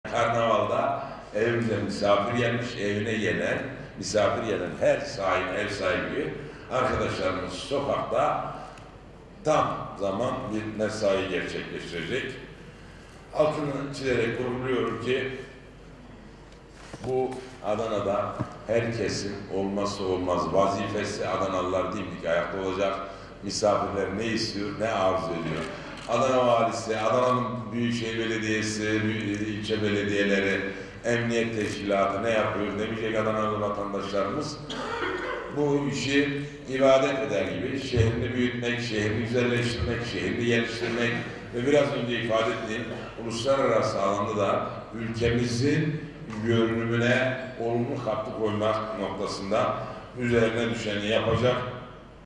Başkan Sözlü : ‘Portakal Çiçeği Karnavalında Herkes İbadet Eder Gibi Çalışacak!..’
Portakal Çiçeği Karnavalı Basın Toplantısında Adana Büyükşehir Belediye Başkanı Hüseyin Sözlü : ‘Bu işi ibadet eder gibi, şehri büyütmek, şehri güzelleştirmek için herkes üzerine düşeni yapacak..’